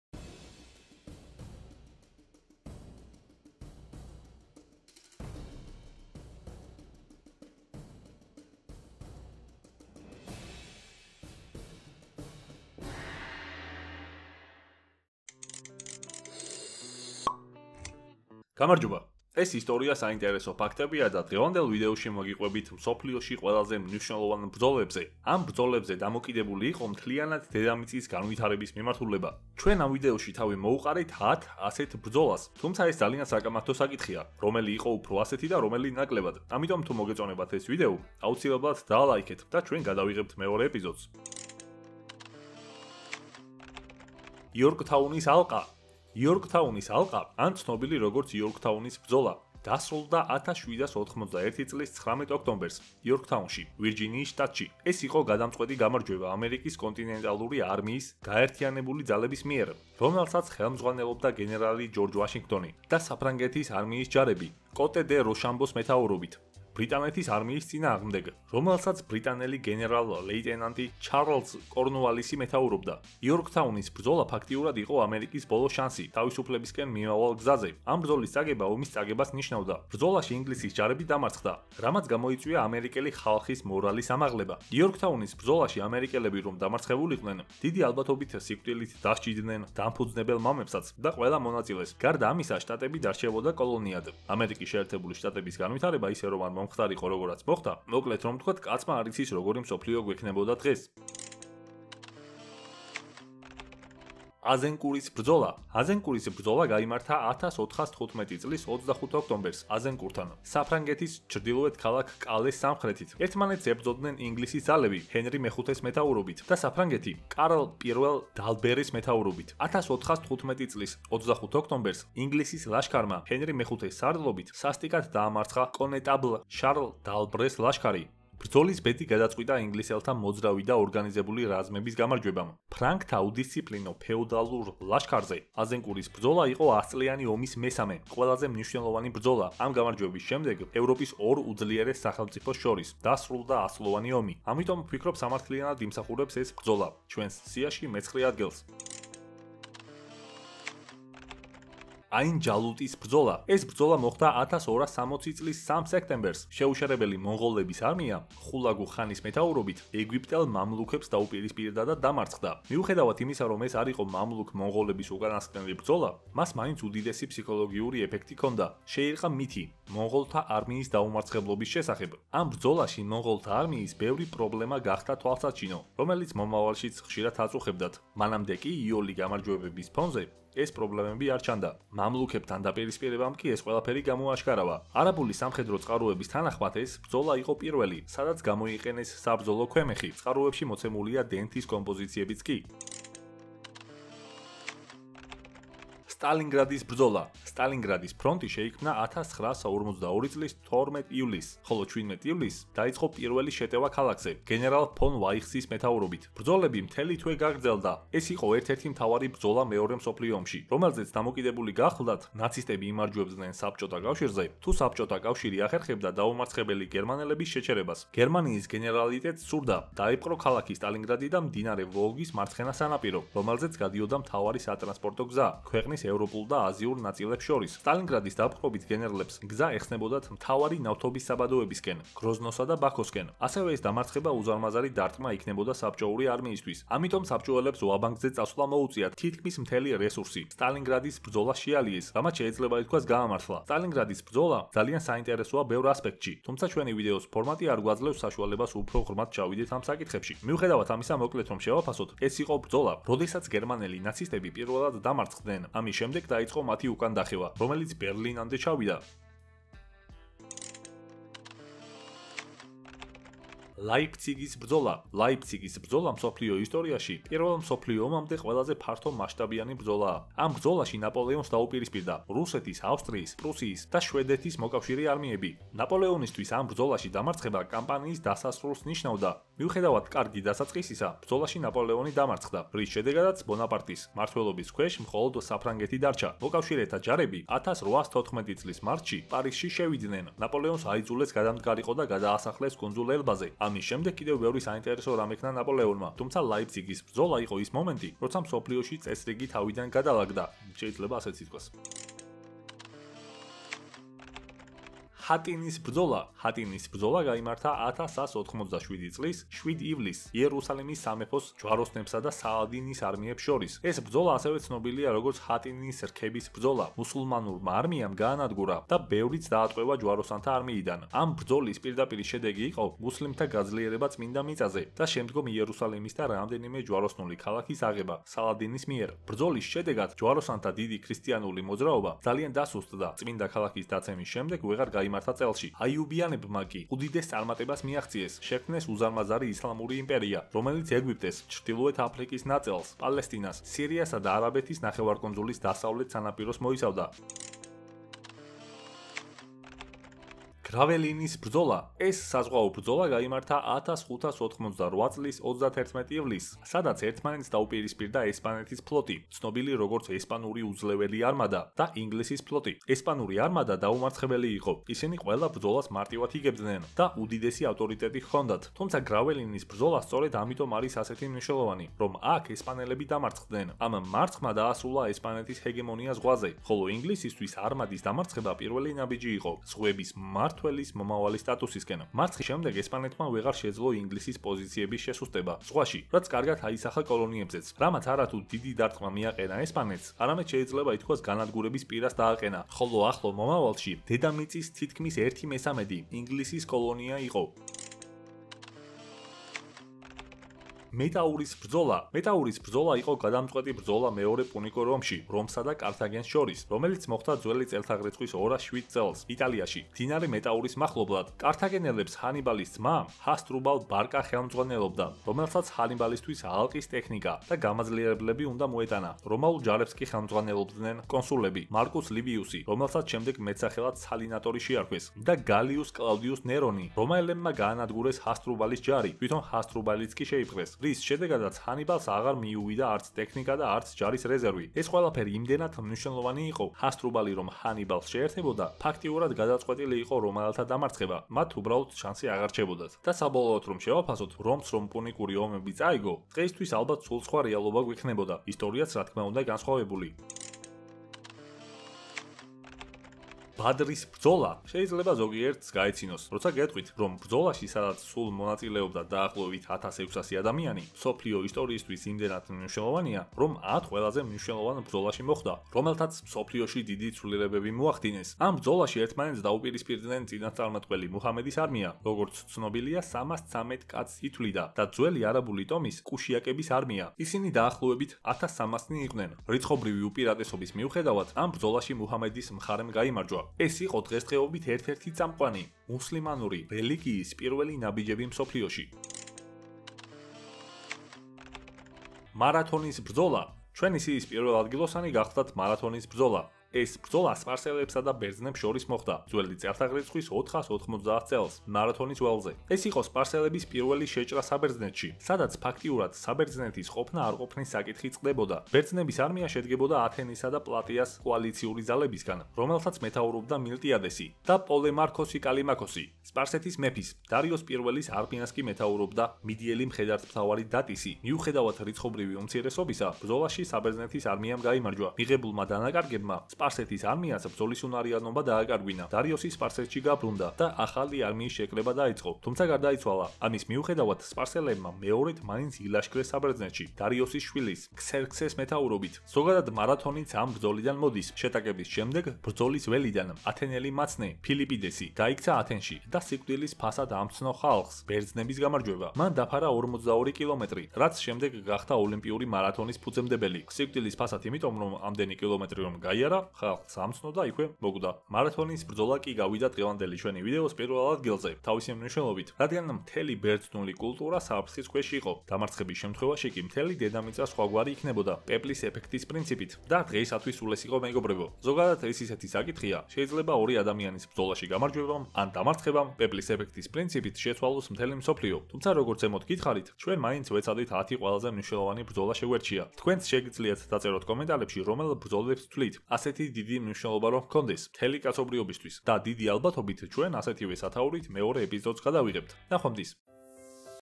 10 ყველაზე მნიშვნელოვანი ბრძოლა კაცობრიობის მანძილზე
10. იორკთაუნის ალყა
9. აზენკურის ბრძოლა
8. აინ ჯალუტის ბრძოლა
7. სტალინგრადის ბრძლა
6. ლაიფციგის ბრძოლა
5. ჰატინის ბრძოლა
4. გრაველინის ბრძოლა
3. მეტაურის ბრძოლა
2. ბადრის ბრძოლა
1. მარათონის ბრძოლა

💟 დონაციის ლინკი.